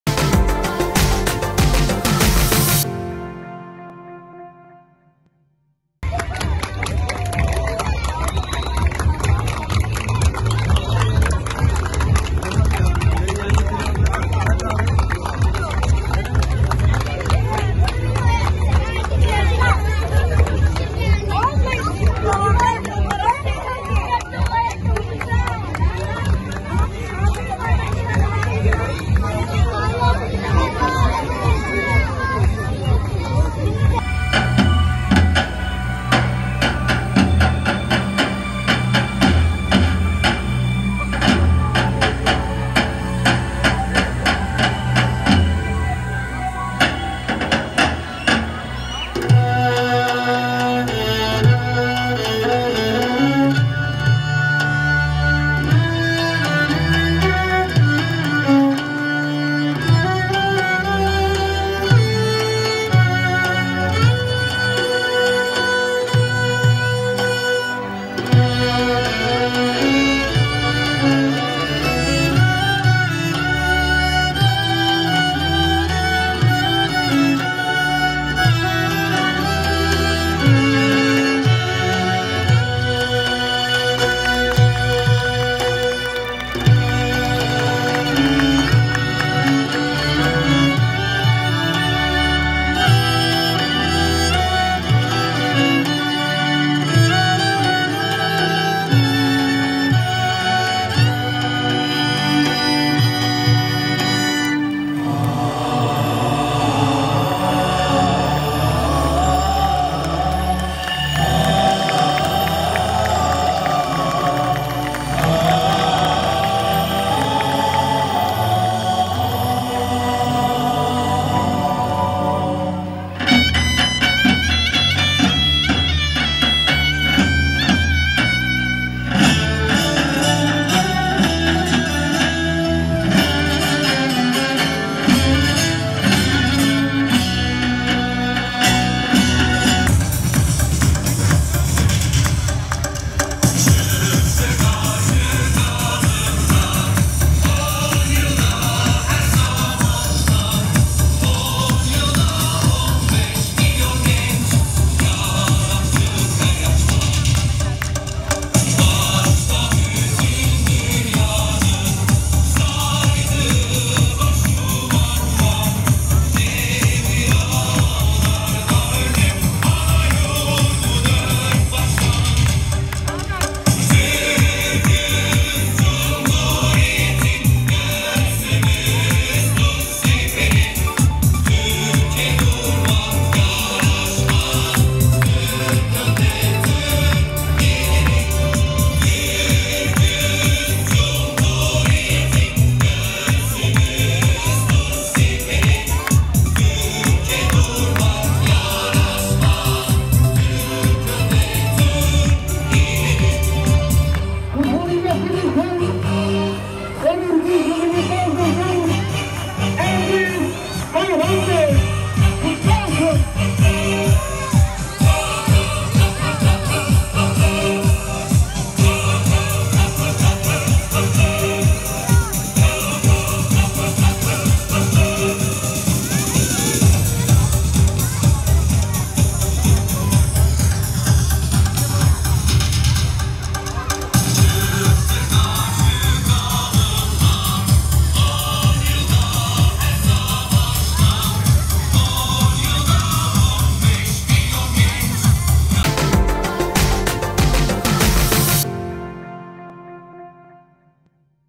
Cumhuriyet Bayramı gösterileri büyük ilgi gördü
Edirne Trakya Birlik İlkokulu öğrencilerinin 29 Ekim gösterileri adeta büyüledi. Trakya Birlik İlkokulu öğrencilerinin hazırladığı Cumhuriyet Bayramı gösterileri büyük ilgi gördü.Cumhuriyetimizin kuruluşunun 100’üncü yıldönümü etkinlikleri kapsamında öğrencilerin Vali Fahri Yücel İlkokulu bahçesinde sunduğu gösteriler beğeniyle izlendi.Trakya Birlik İlkokulu Müdür Yardımcısı Gürkan Büyükharcan’ın zeybek oyunu ise büyük beğeni topladı.